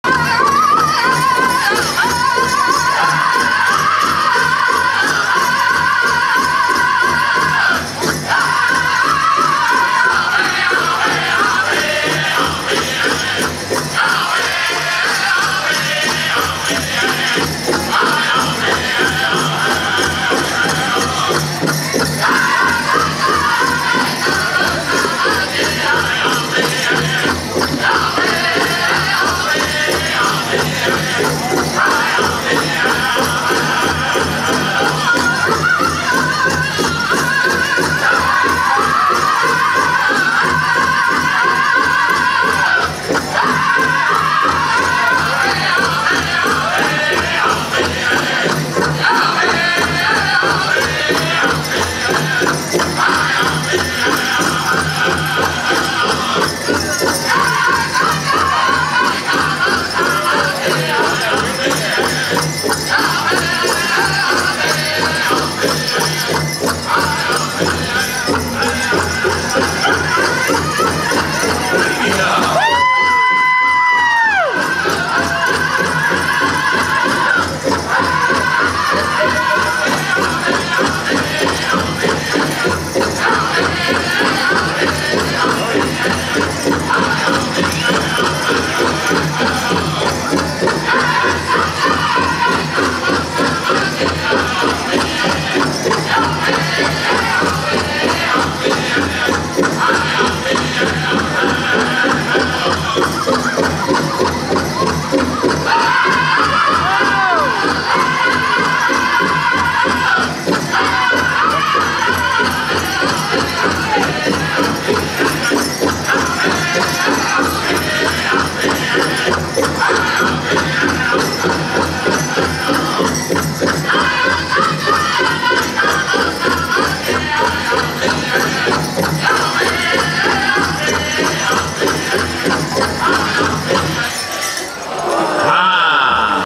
Boys Grassdance Special Finalist Song 1 in Mandaree July 16, 2017
Sunday Funday Boys Grass Dance Special at Mandaree Celebration 2017 July 14 - 16, 2017 in Mandaree, North Dakota